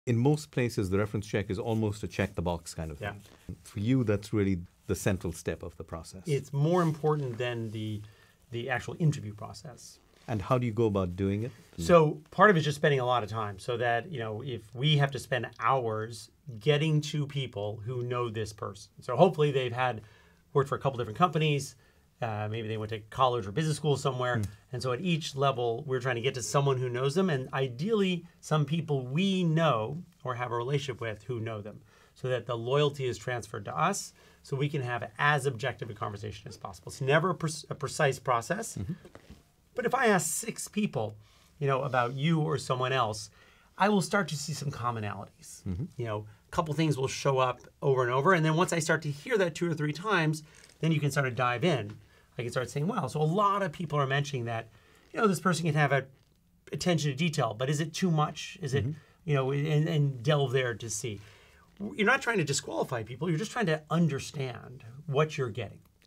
Kevin Ryan Shares Why Reference Interviews Matter More than Traditional Interviews
Most people treat reference checks as a formality in the interview process. Kevin Ryan, founder of AlleyCorp and serial entrepreneur, shares why taking time to search for and speak with references can provide the most valuable insights into a candidate.